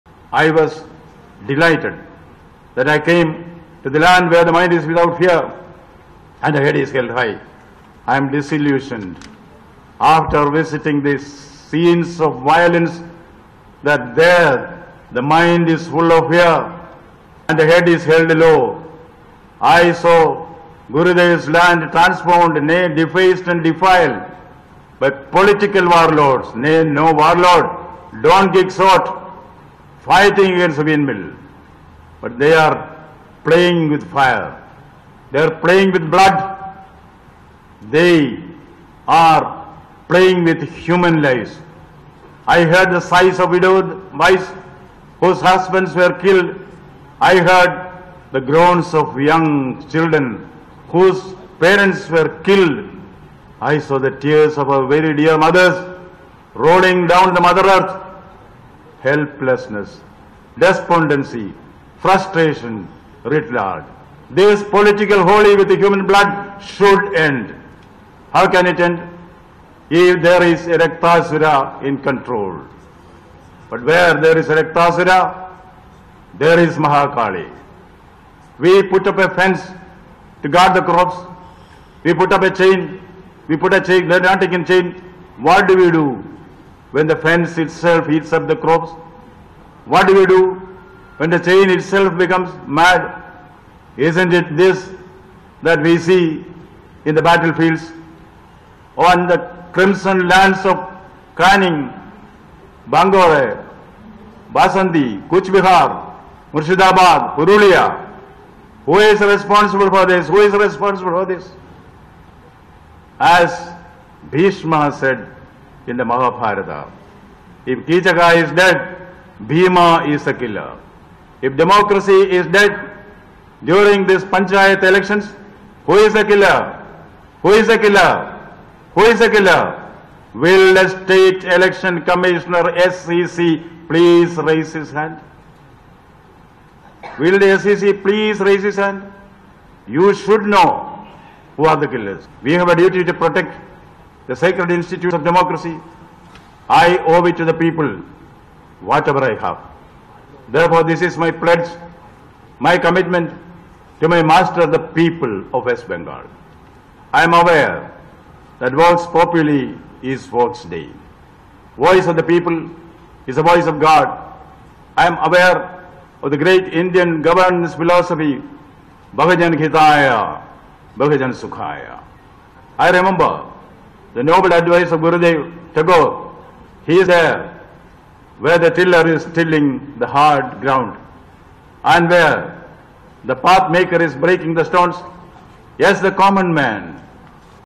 রাজ্যের গণতন্ত্রকে হত্যা করা হয়েছে, কমিশনার দায়িত্ব পালনে ব্যর্থ: রাজ্যপাল
পঞ্চায়েত নির্বাচনে রাজ্যের গণতন্ত্রকে হত্যা করা হয়েছে, কমিশনার দায়িত্ব পালনে ব্যর্থ: রাজ্যপাল